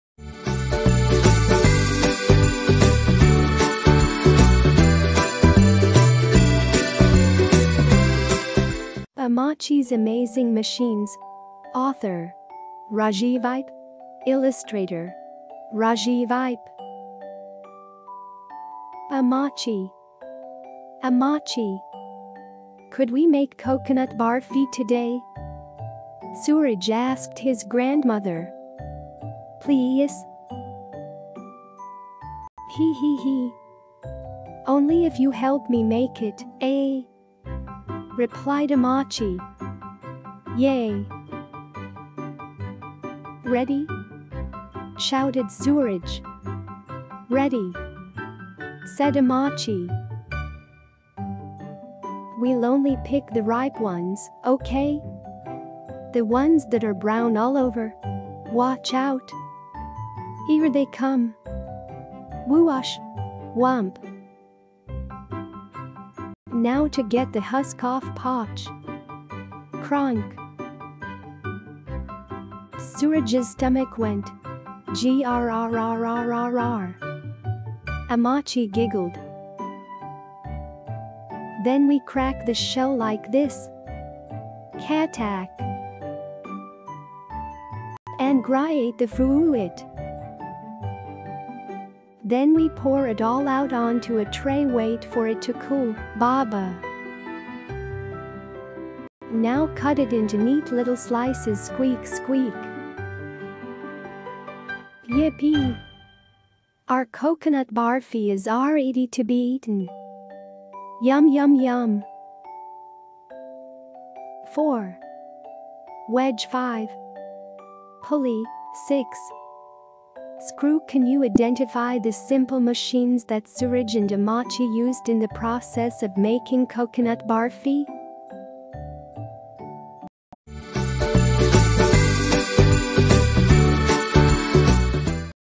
[Vibo4Kids]Ammachi's Amazing Machines
Ammachi's Amazing Machines
Author: Rajiv Eipe
Illustrator: Rajiv Eipe
"Ammachi!
Ammachi!
Could we make coconut barfi today?"
Sooraj asked his grandmother.
"PLEEEEEAASE?"
"Hee hee hee!
Only if you help me make it, eh?"
replied Ammachi.
"YAY!"
"Ready?"
shouted Sooraj.
"Ready!"
said Ammachi.
"We’ll only pick the ripe ones, okay?
The ones that are brown all over...
Watch out!
Here they come!"
WHOOOSH...
WHUMP!
"Now to get the husk off…" POTCH!
CRAANKK!
Sooraj’s stomach went: "GRRRRRR!"
Ammachi giggled.
"Then we  crACK the shell…  like THIS!"
KHATAK!
"And grrrrrraaaaate the frrrrrrrruuuit!"
"Toss it into the pan with all the other ingredients…" "It smells SO good , Ammachi!"
"Then we pour it all out on to a tray… Wait for it to cool, baba!"
"Now cut it into neat little slices…" SQUEAK SQUEAK
"YIPPEE!
Our coconut barfi is r eady to be eaten!"
Ammachi's barfi was made using SIMPLE MACHINES!
Simple machines make it easier to do work.
There are six kinds of simple machines: 1.
Inclined plane 2.
Lever 3.
Wheel and Axle
4.
Wedge 5.
Pulley 6.
Screw Can you identify the simple machines that Sooraj and Ammachi used in the process of making coconut barfi?